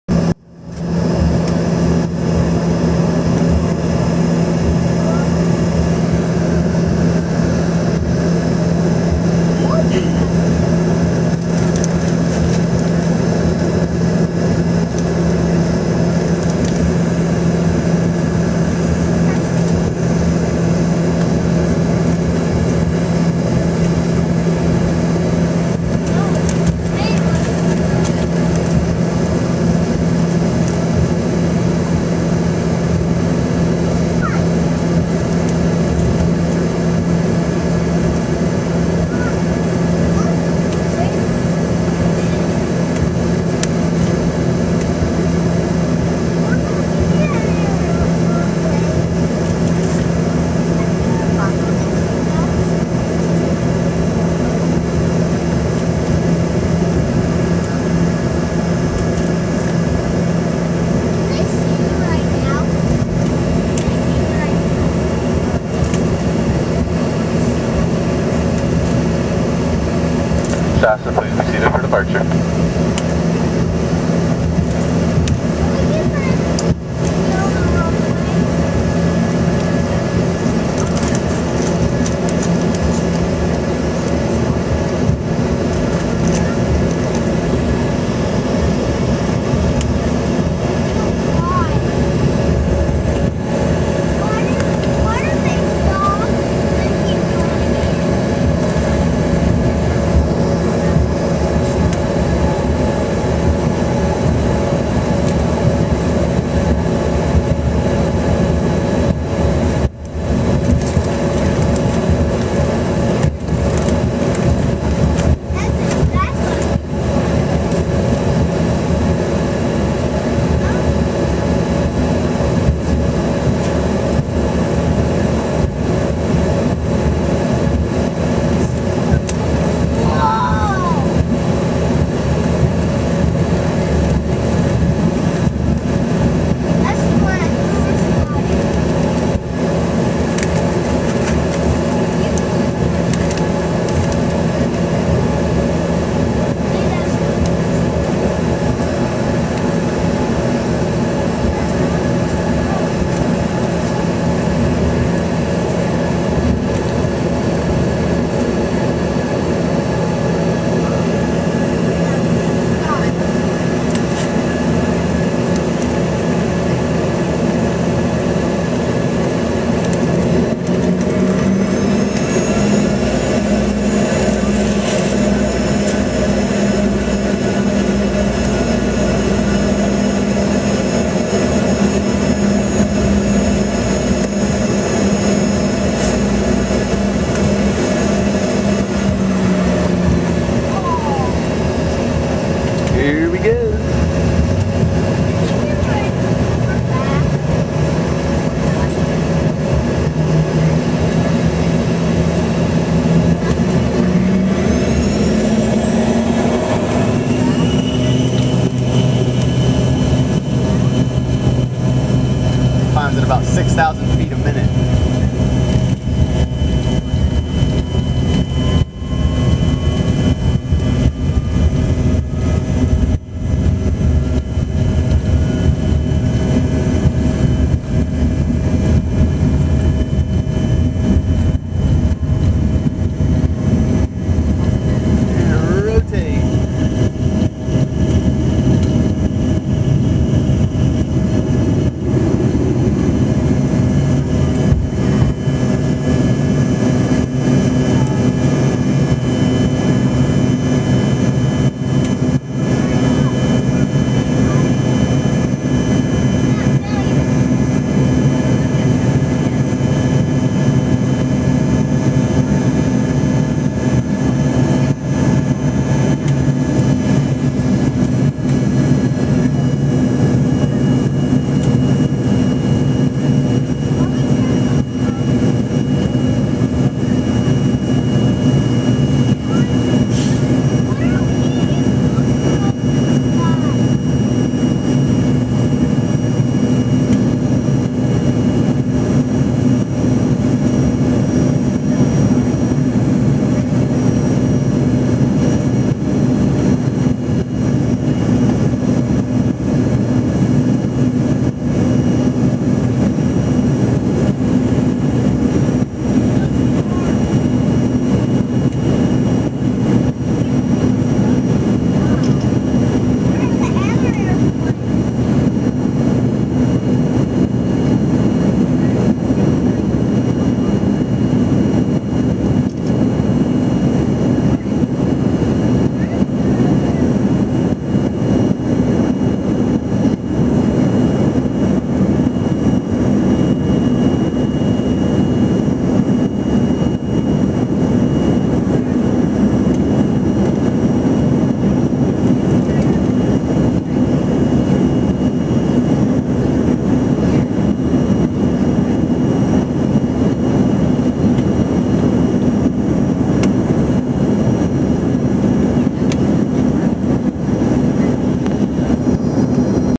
KICT taxi and takeoff into overcast clouds 5500 msl
Flight from KICT to Houston Bush intercontinental airport.